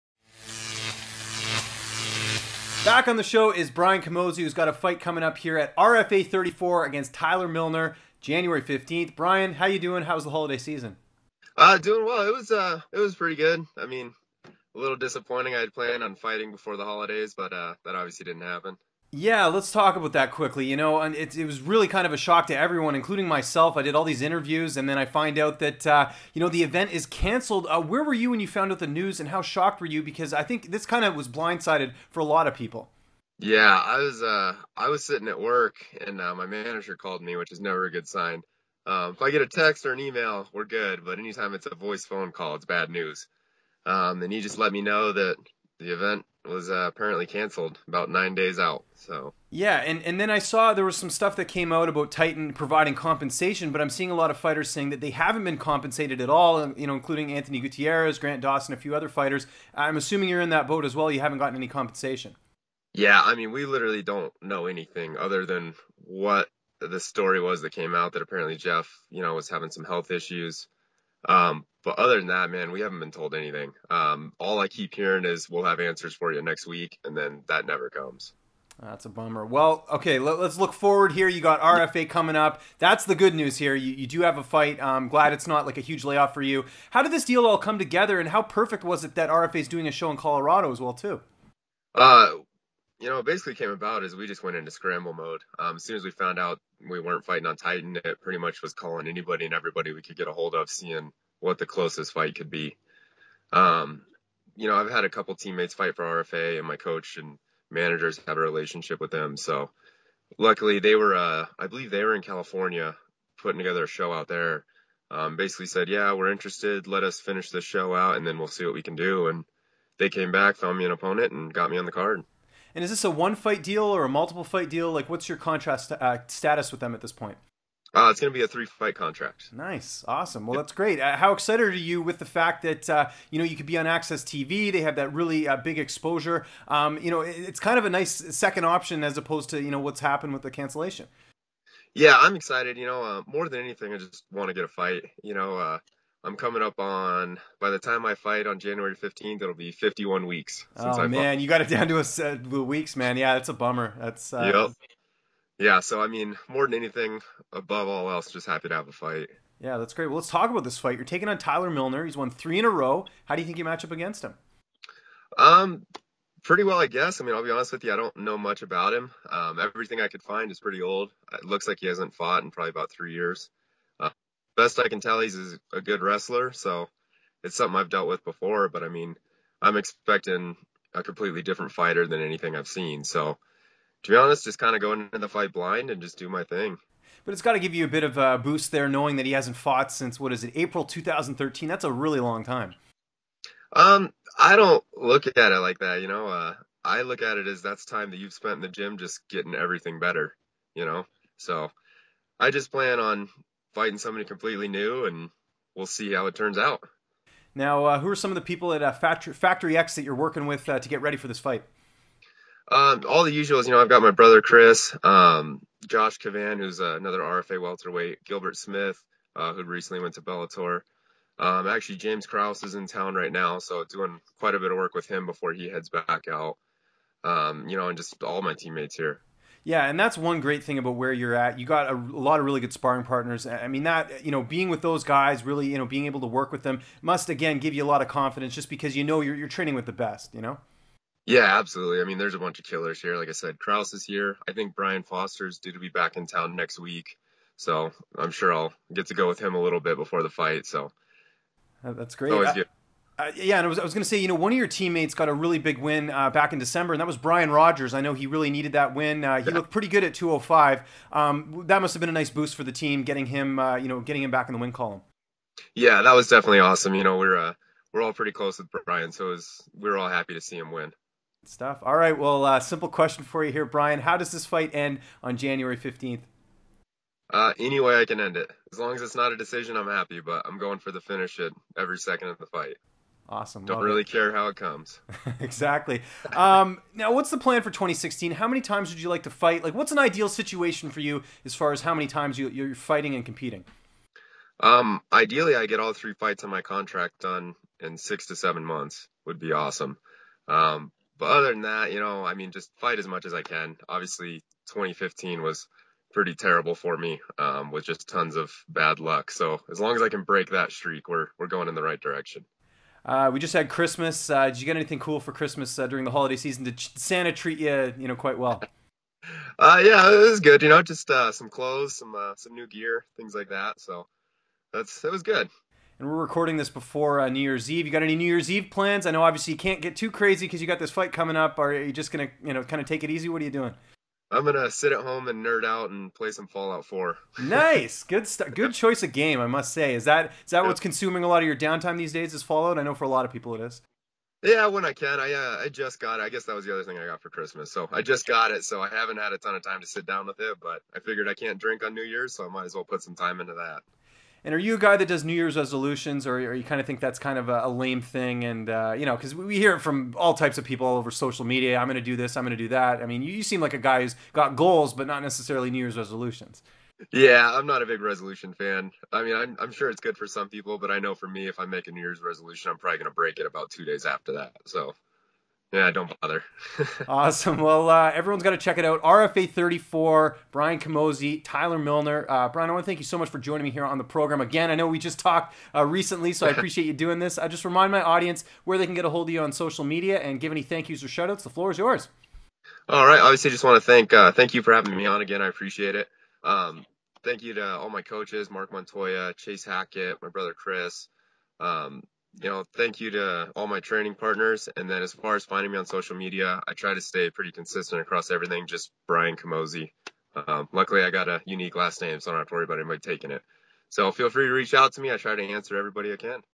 Brian Camozzi talks RFA debut, Titan FC 36 being cancelled & 2016 plans
Welterweight Brian Camozzi (4-2) discusses his upcoming fight with Tyler Milner at RFA 34 on Jan. 15th

Brian also discusses Titan FC 36 being cancelled, how this deal with RFA came together and his outlook for 2016.